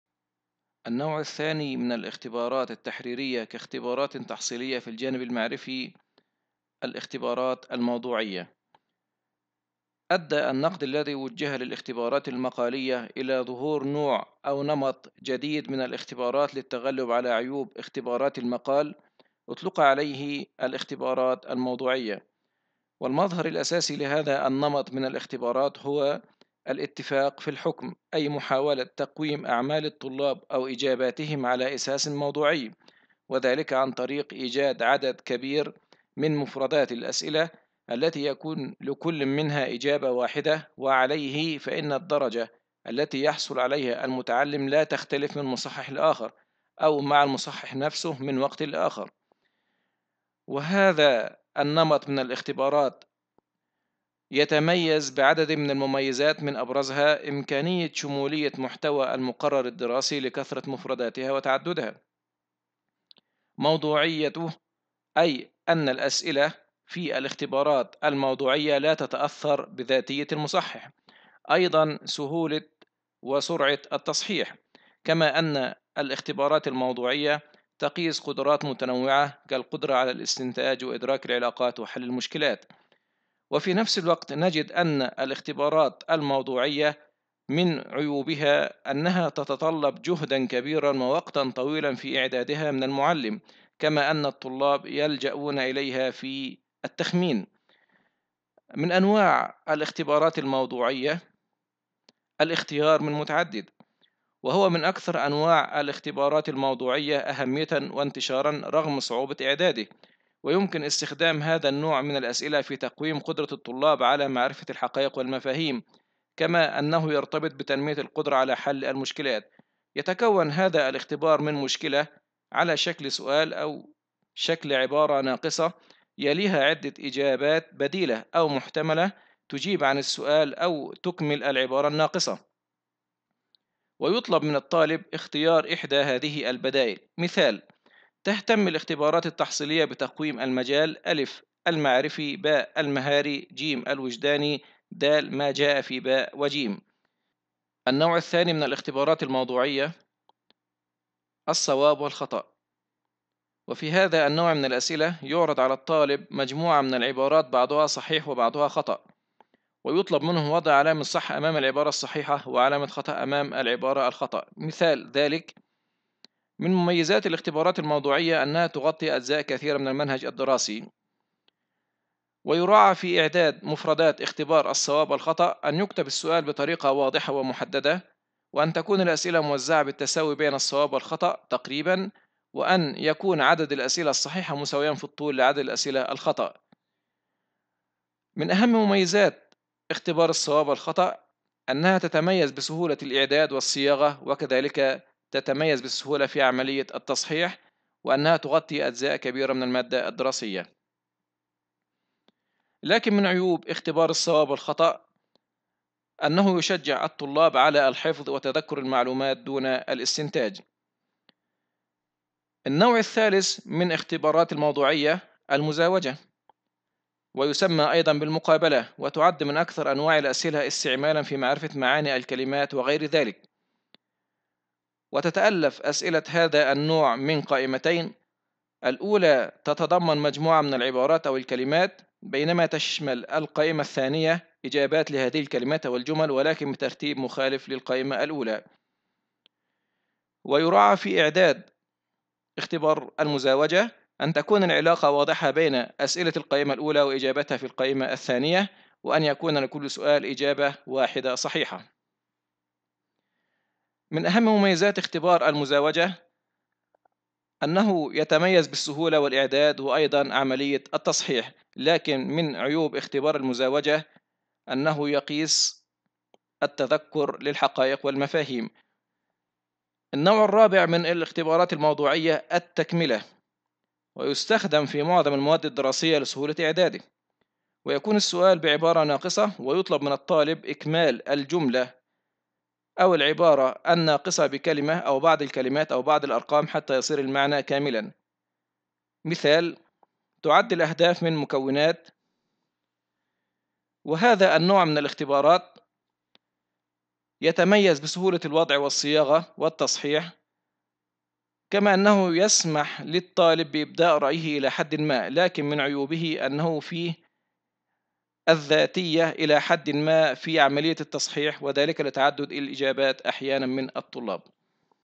أسس المناهج - الوحدة 8 : أساليب التقويم المجال المعرفي - 2
المقررات المفتوحة
أسس المناهج - 471 نهج-2
الوحدة الثامنة - التقويم
1. اختبارات الاختيار من متعدد